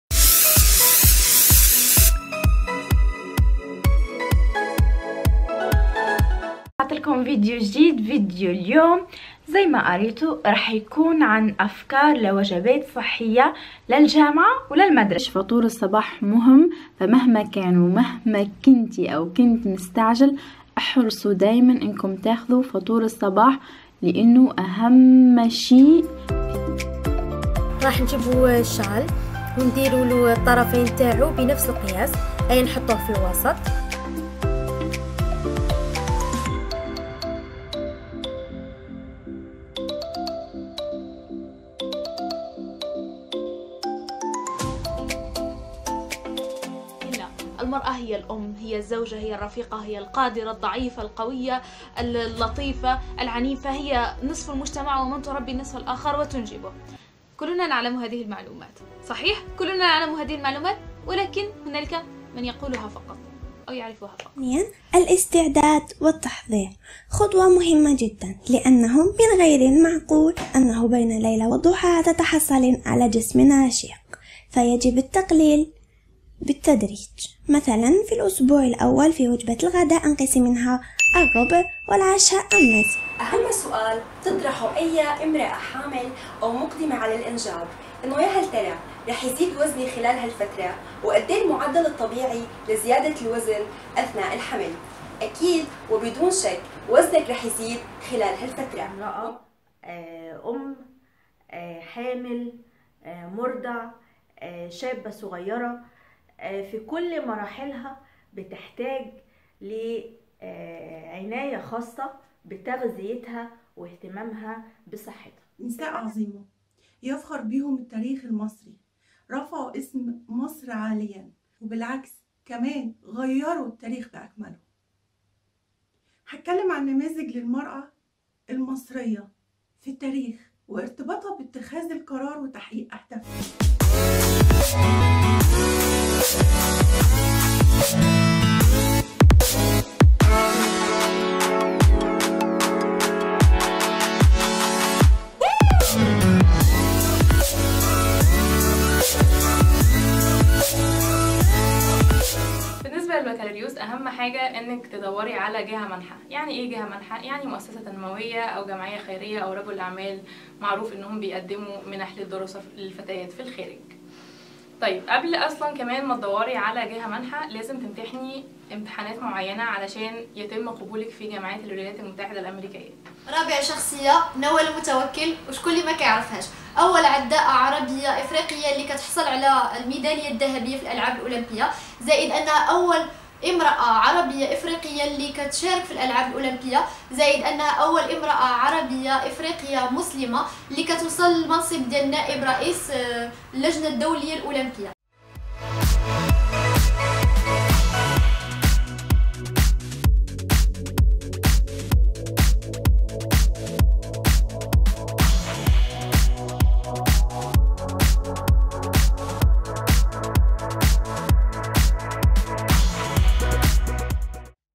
حقائق لا تعرفها عن المرأة العربية... لن تصدق 😯
حقائق لا تعرفها عن المرأة العربية، اكتشفوها في عشرة فيديوهات رائعة في الدورة الثالثة من أسبوع يوتيوب المرأة العربية على بودكاست آرابيا، 10 متأهلات من 5 دول عربية هي تونس، المغرب، الجزائر، مصر وسوريا يتنافسن على جائزة 1000 دولار. وهن:
Soha Zaid World،  wiemaxuxi، In line with Tima، Djihane Babori، ماما مملكه، وآخريات
انطلاق التصويت سيكون يوم الخميس المقبل